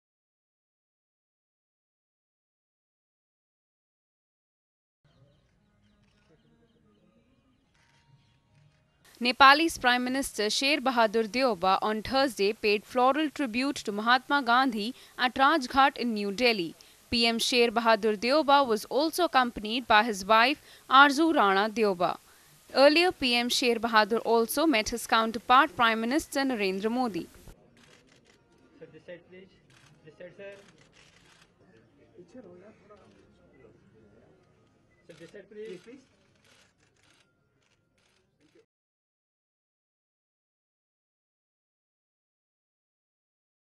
Nepal PM Sher Bahadur Deuba pays tribute to Mahatma Gandhi at Rajghat - ANI News
New Delhi, Aug 24 (ANI): Nepalese Prime Minister Sher Bahadur Deuba on Thursday paid floral tribute to Mahatma Gandhi at Rajghat in New Delhi.